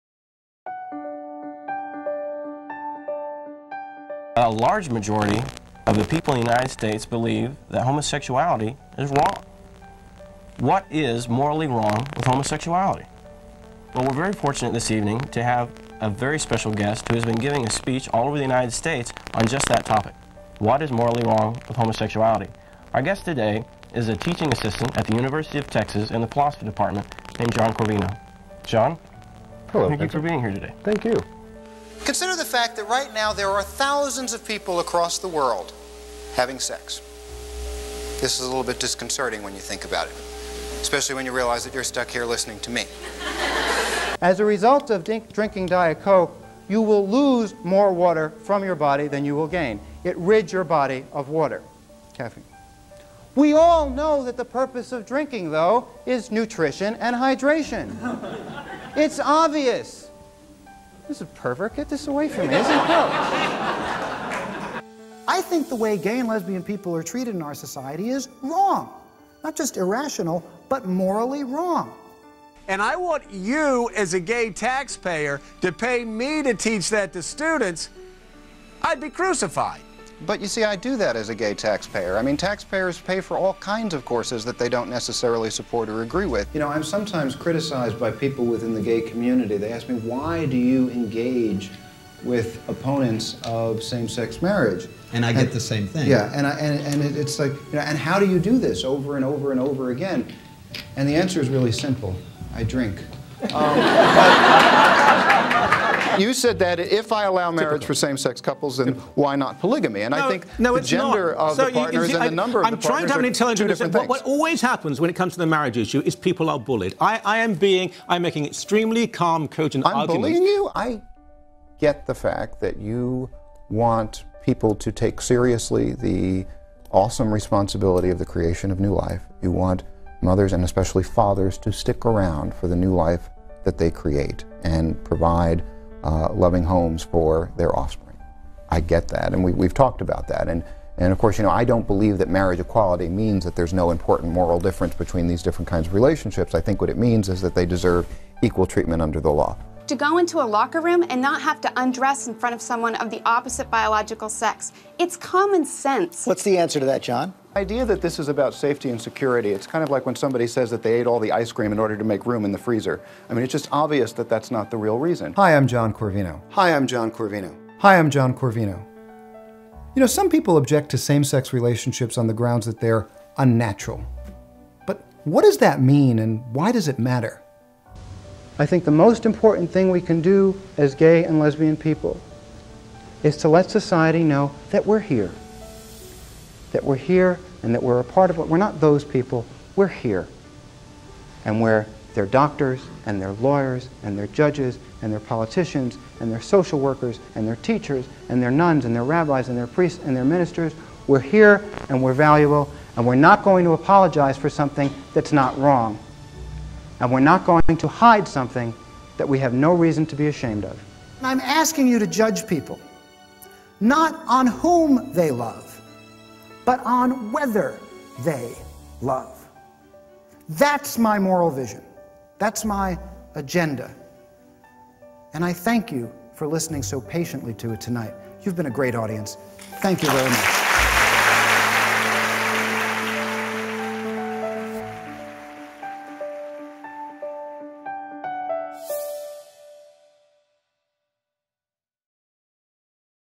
John Corvino: 25 Years
To commemorate 25-year anniversary of his lecture "What's Morally Wrong with Homosexuality?" and his work for equality more generally, John Corvino was honored with the inaugural “Community Hero Award” at the Affirmations Spring Bash in Detroit on March 18, 2017. This compilation video was shown at the event. Edited by Christian Hendricks.